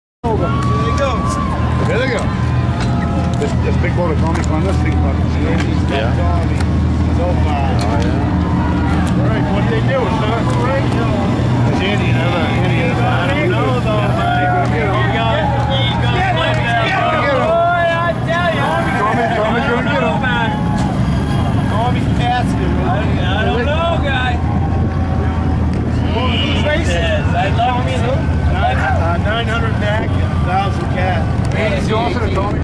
2010 Harpswell Lobster Boat Races MOTIVATION & WHISTLIN DIXIE
Don't know who won cause i wasn't on the finish line but it looked like a very good race!!